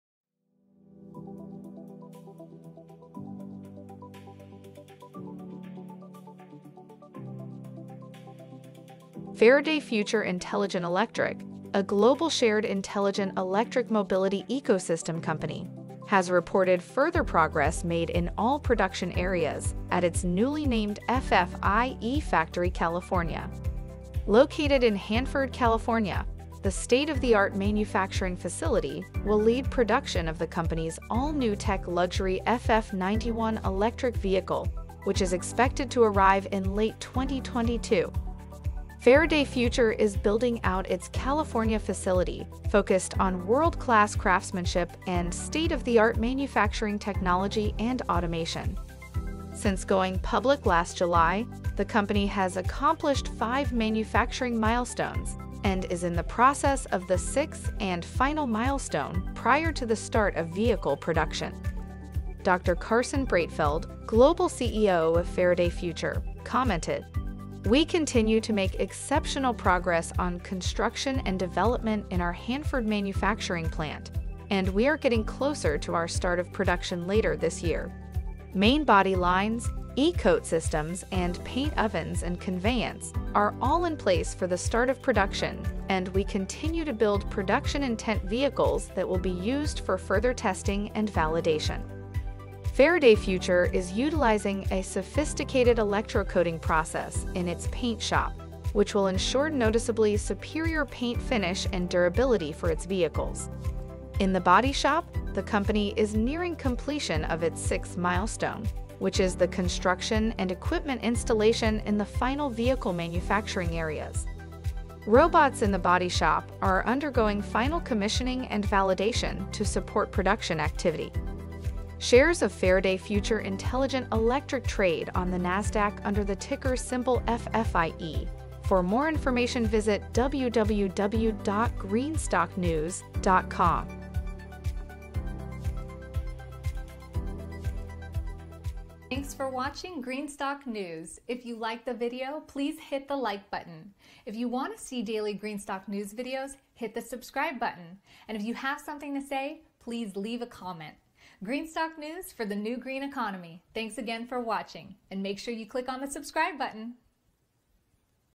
Faraday Future ($FFIE) Reports Further Progress at its Newly Named FF ieFactory California Facility
Faraday Future Intelligent Electric ($FFIE), a global shared intelligent electric mobility ecosystem company, has reported further progress made in all production areas at its newly named FF ieFactory California. Located in Hanford, California, the state-of-the-art manufacturing facility will lead production of the company’s all-new TechLuxury FF 91 electric vehicle, which is expected to arrive in late 2022.

Faraday Future is building out its California facility focused on world class craftsmanship and state-of-the-art manufacturing technology and automation.Since going public last July, the company has accomplished five manufacturing milestones and is in the process of the sixth and final milestone prior to the start of vehicle production.

Dr. Carsten Breitfeld, Global CEO of Faraday Future commented: “We continue to make exceptional progress on construction and development in our Hanford manufacturing plant, and we are getting closer to our start of production later this year. Main body lines, e-coat systems and paint ovens and conveyance are all in place for the start of production and we continue to build production-intent vehicles that will be used for further testing and validation.”

Faraday Future is utilizing a sophisticated electrocoating process in its paint shop, which will ensure noticeably superior paint finish and durability for its vehicles. In the body shop, the company is nearing completion of its 6th milestone, which is the construction and equipment installation in the final vehicle manufacturing areas. Robots in the body shop are undergoing final commissioning and validation to support production activity.

Shares of Faraday Future Intelligent Electric trade on the NASDAQ under the symbol FFIE.